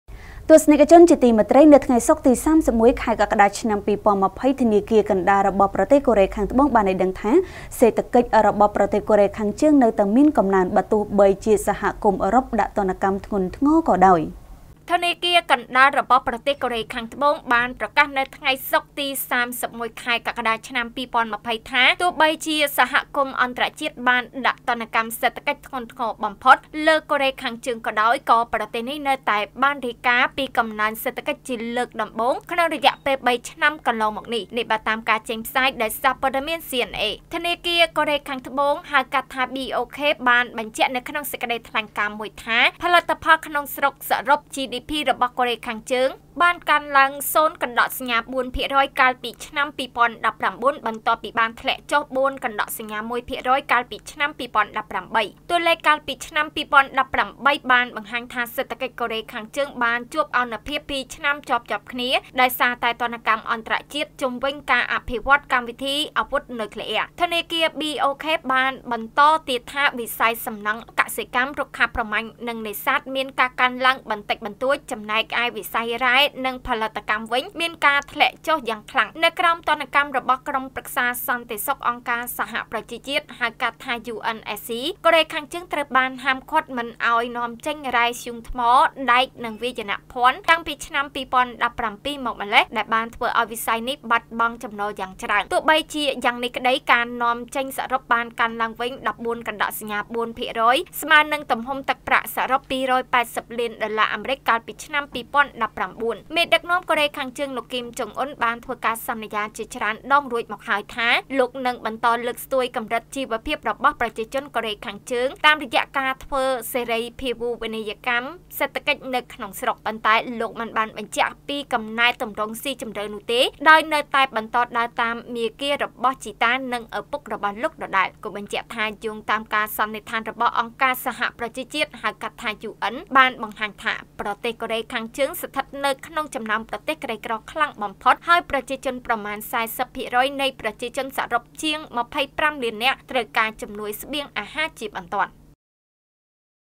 កូរ៉េខាងជើងនៅតែមានកំណើនសេដ្ឋកិច្ច ទោះបីទទួលរងទណ្ឌកម្មពីអន្តរជាតិ, នេះបើតាមធនាគារកូរ៉េខាងត្បូង
* រាយការណ៍ព័ត៌មានដោយកញ្ញា  យឹម ចាន់ស្រីវីន និងអ្នកនាង កេត សត្យា, ទូរទស្សន៍ព័ត៌មាន Fresh News